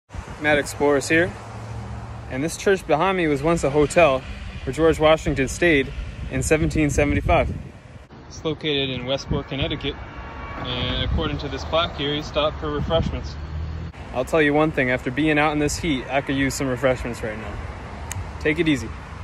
Revolutionary War Hotel
Visiting the site in my town where George Washington stopped for drinks in 1775